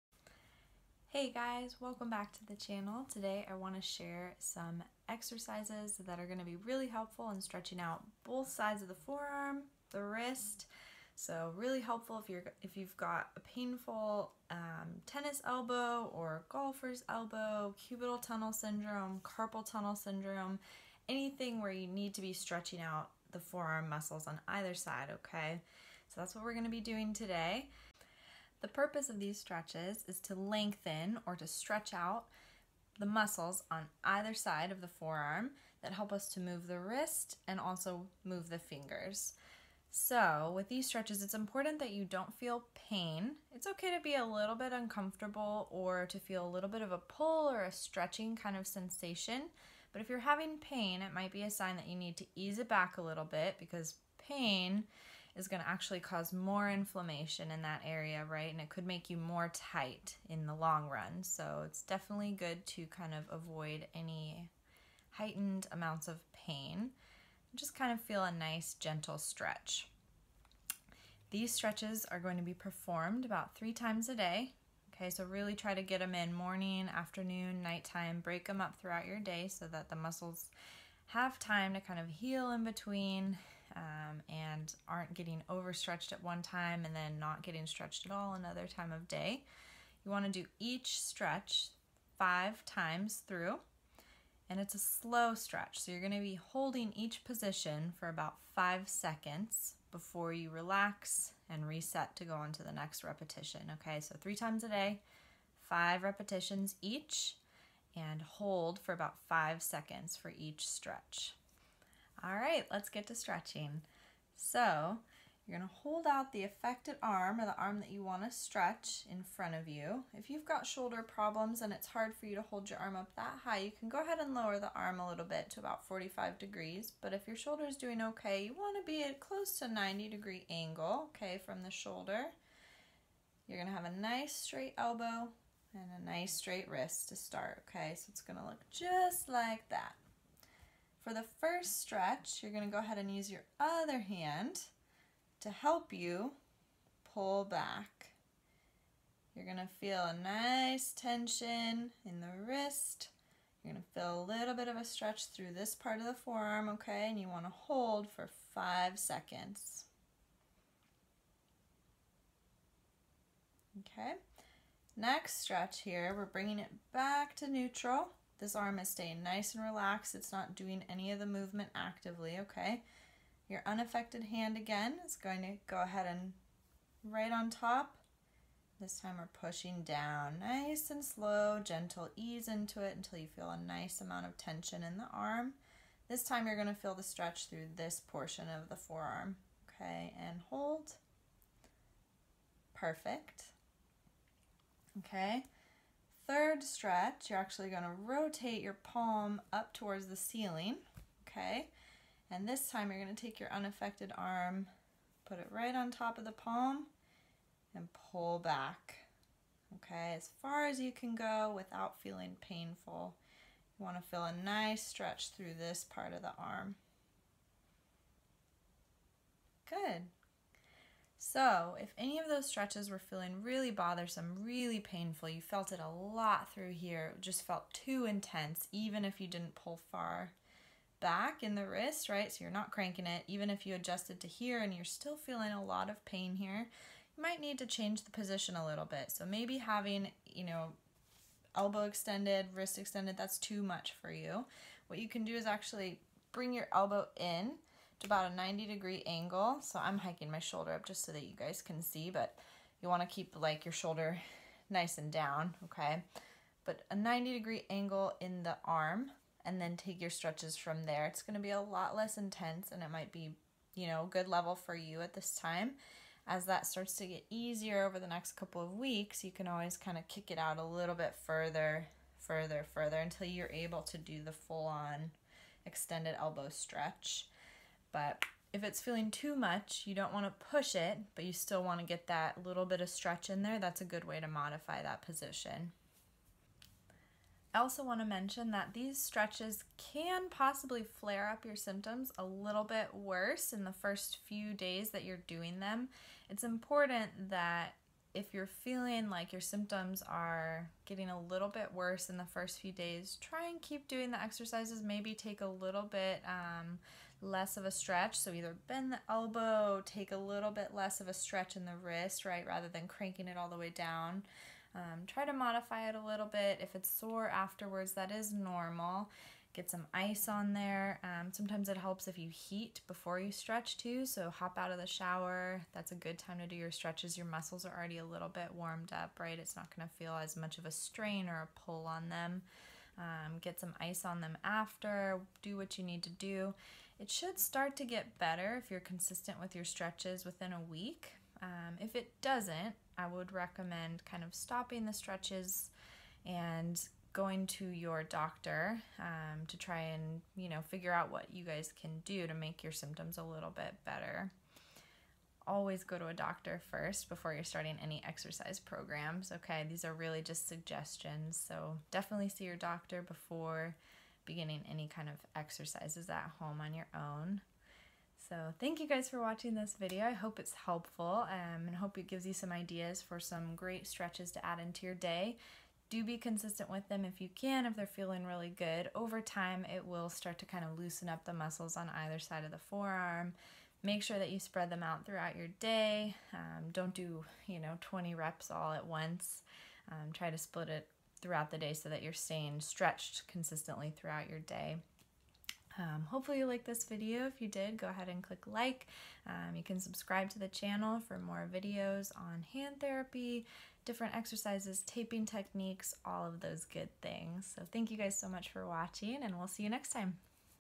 3 Easy Stretches for Tennis Elbow + Elbow Pain
Kelly demonstrates 3 easy stretches that can relieve symptoms of tennis elbow, golfer's elbow, cubital tunnel syndrome, and more! Start treating your elbow pain today.